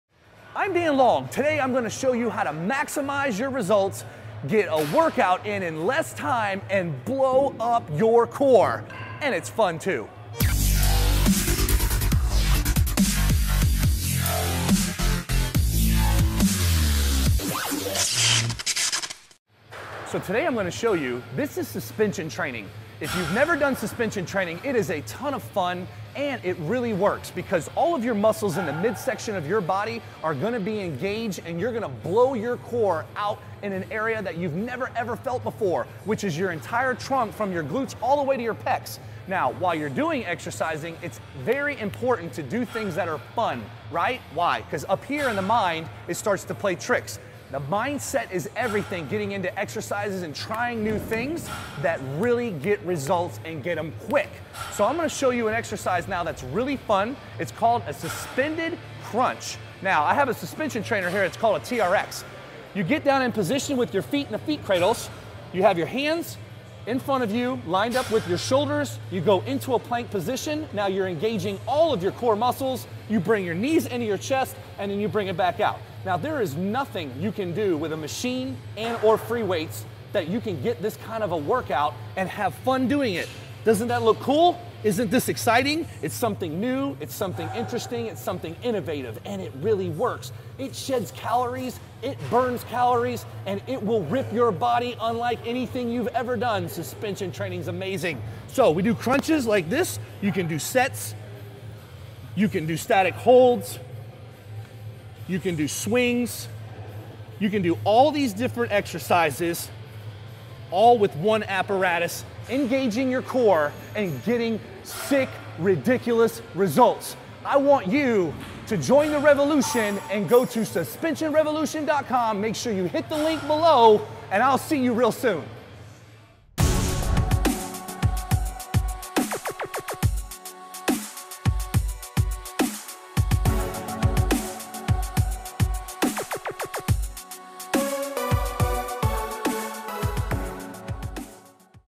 Dan Long TRX-Quick Results With These Tips!
Dan Long shows you how fast, fun, and easy it is to get
quick sick ridiculous results with his Suspension Revolution
program. Now available online for easy access.